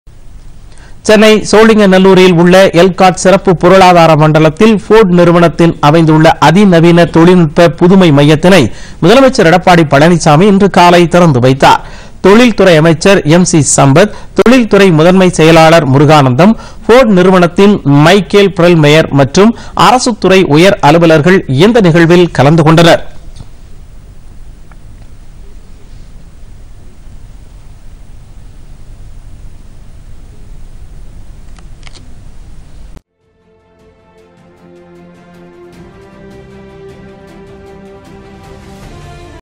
ஃபோர்டு தொழில்நுட்ப மையம் திறப்பு
சென்னை சோழிங்கநல்லூரில் ஃபோர்டு நிறுவனத்தின் அதிநவீன  தொழில்நுட்ப மையம் - இன்று திறந்துவைத்தார் முதலமைச்சர் எடப்பாடி பழனிசாமி.